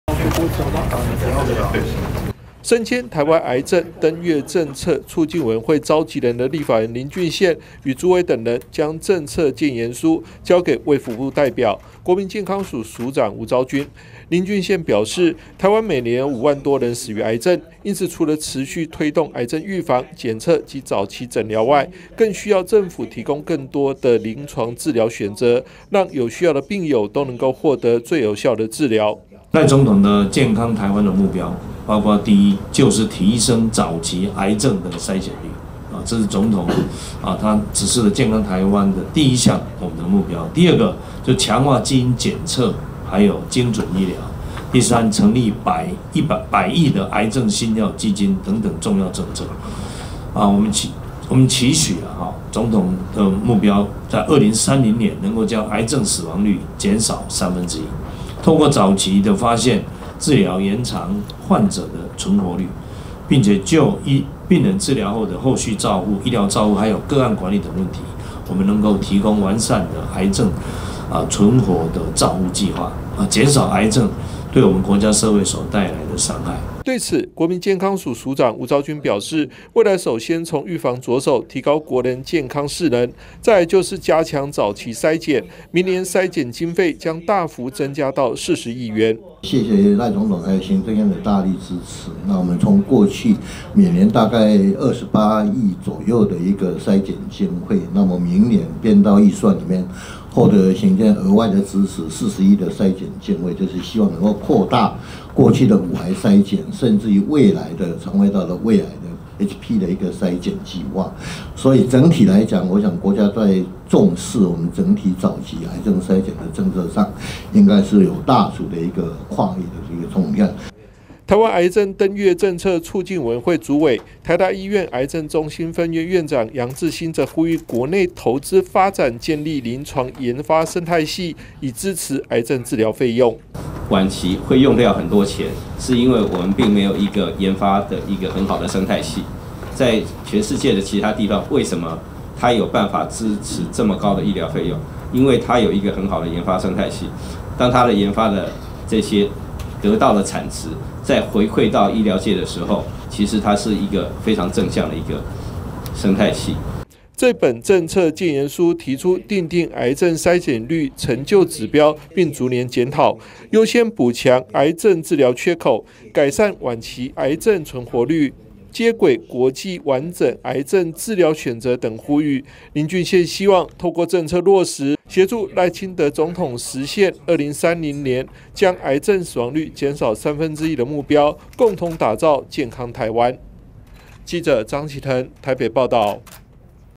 癌症 死亡率|賴清德 健康台灣|林俊憲 登月計畫|立法院 厚生會|篩檢 早期|2024 中嘉新聞|落實癌症登月計畫 降癌症死亡率 林俊憲攜手專家發布政策建言書
降低癌症死亡率 林俊憲盼落實登月計畫 實現賴清德總統2030年目標

（中嘉新聞記者張啓騰/台北中正區報導）
癌症是國人十大死因之首，根據統計，台灣每年有五萬多人因為癌症死亡，立法委員林俊憲因此發起成立台灣癌症登月政策促進委員會，攜手專家學者提出政策建言書，希望協助政府改善癌症的預防、檢測、診斷與控制，讓有需要的癌友都能獲得最有效的治療，協助總統賴清德實現在2030年將癌症死亡率減少三分之一的目標。

身兼台灣癌症登月政策促進委員會召集人的立法委員林俊憲與主委等人將政策建言書交給衛福部代表-國民健康署署長吳昭軍，林俊憲表示台灣每年有5萬多人死於癌症，因此除了持續推動癌症預防、檢測及早期診斷外，更需要政府提供更多的臨床治療選擇，讓有需要的病友都能夠獲得最有效的治療。

對此國民健康署署長吳昭軍表示未來首先從預防著手，提高國人健康識能，再來就是加強早期篩檢，明年篩檢經費將大幅增加到40億元。

台灣癌症登月政策促進委員會主委-台大醫院癌症中心分院院長楊志新則呼籲國內投資發展建立臨床研發生態系以支持癌症治療費用。

這本政策建言書提出訂定癌症篩檢率成效指標並逐年檢討，優先補強癌症治療缺口、改善晚期癌症存活率、接軌國際完整癌症治療選擇等呼籲，林俊憲希望透過政策落實，協助賴清德總統實現2030將癌症死亡率減少三分之一的目標，共同打造健康台灣。